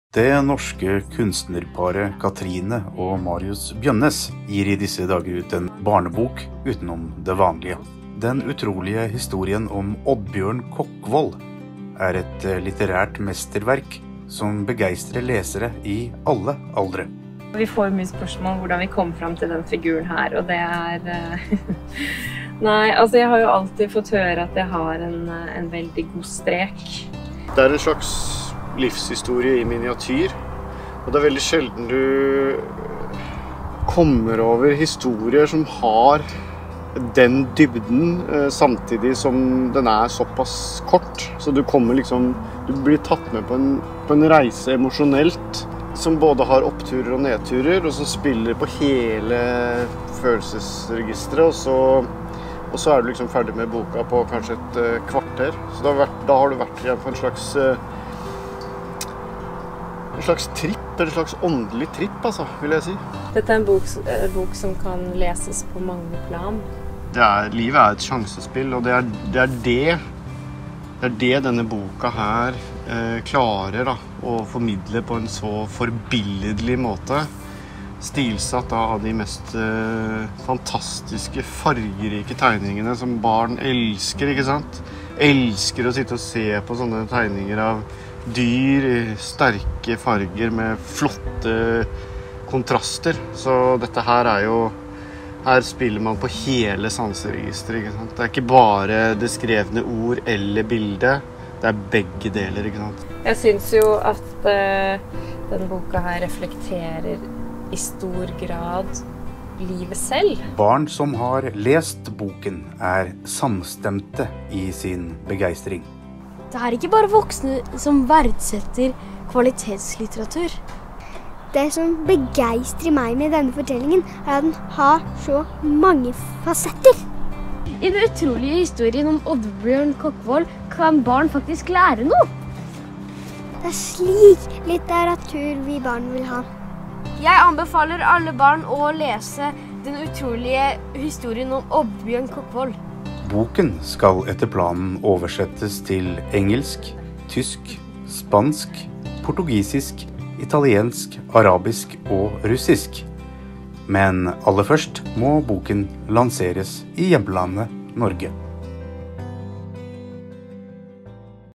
Promovideo "Den Utrolige Historien om Oddbjørn Kokkvold"
Ekteparet Kathrine og Marius Bjønness lanserer barneboken "Den Utrolige Historien om Oddbjørn Kokkvold", en delvis sann historie om livet og alt som kan skje mens man lever det. Her kan barn lære at det er ikke alltid prinsen finner prinsessen, og at lykke kun er midlertidig.